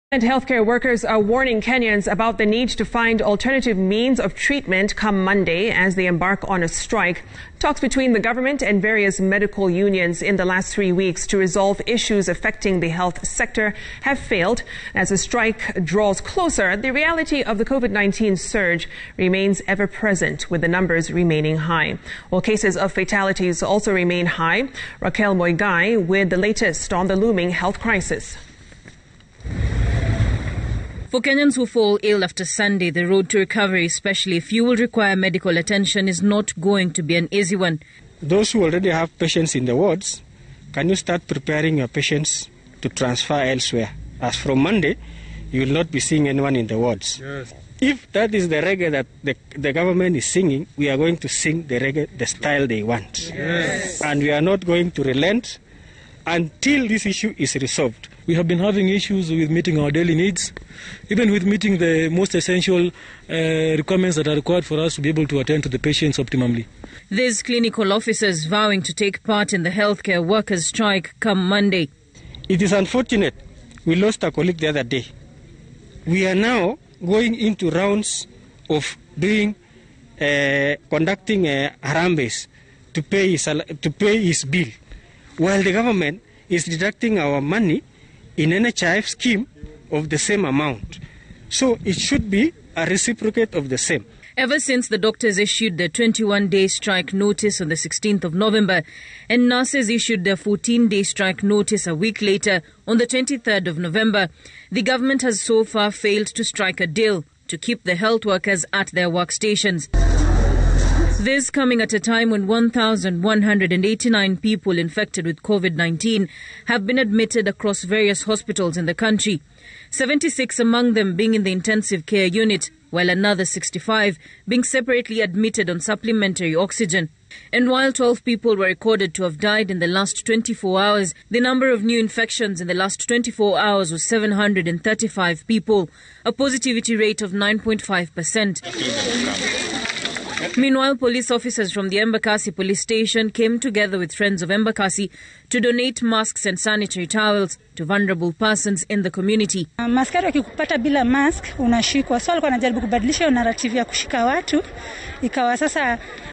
Crisis as health workers vow to down tools
Doctors, nurses, clinicians, lab technicians strike begins Monday
Ngetich: Those with patients in wards should move them elsewhere
735 new infections reported in the past 24 hours
Total reported cases in Kenya rise to 87,984
12 patients succumb to the virus in the last 24 hours pushing death toll to 1,518